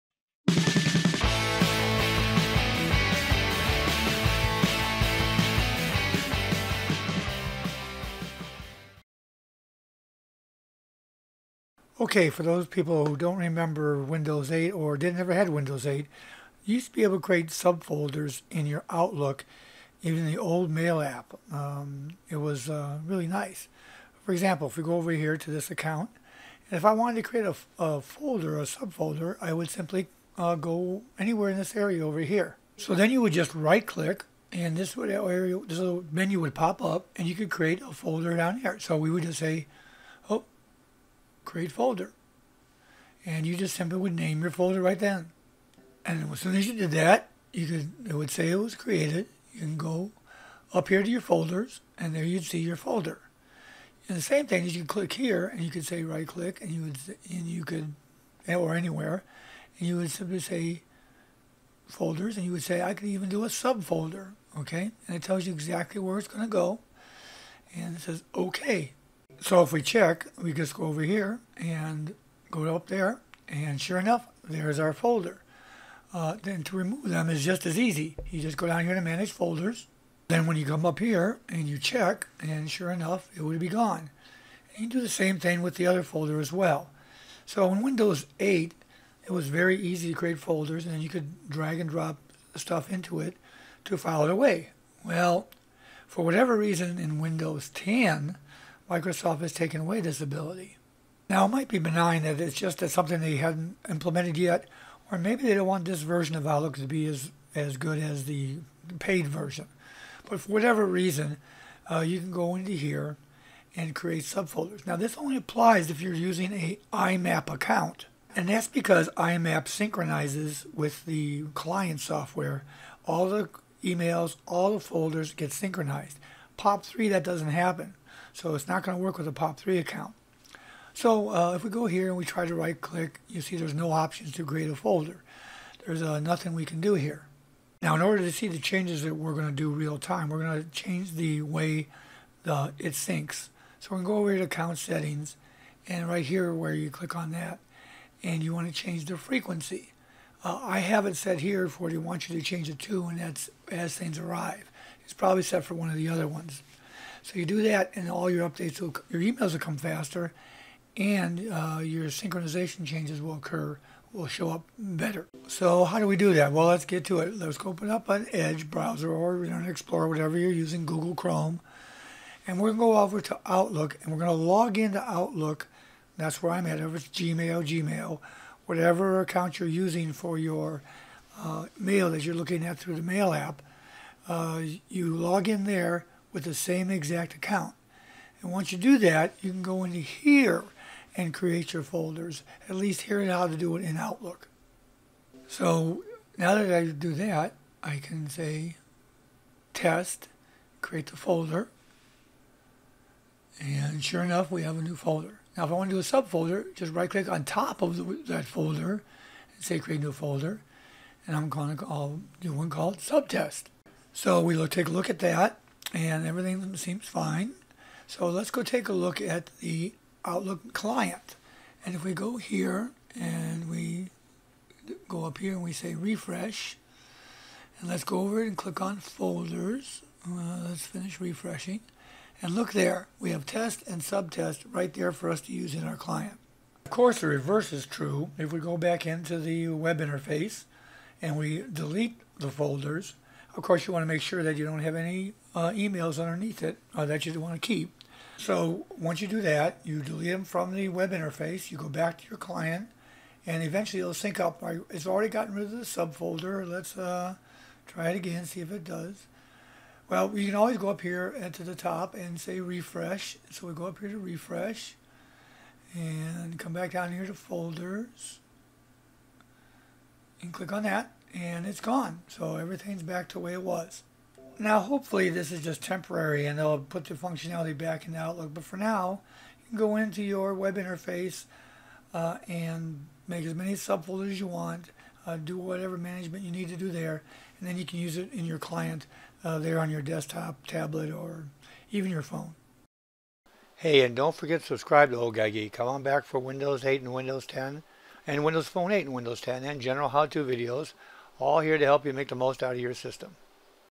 Windows 10 Outlook App - Create Folders (Click Upper Right to See Updated Version)
UPDATE:  The Windows 10 Mail App has been updated with the ability to make folders right from within the app.  Here's the link to the new video:

In Windows 10, the Outlook App replaced the Mail App in Windows 8.  But the ability to create mail folders and subfolders disappeared.  But if you are using an IMAP account, there is a way to create the folders.

Hopefully this is a temporary situation and Microsoft will put back the ability to create folders in the Outlook client that comes with Windows 10.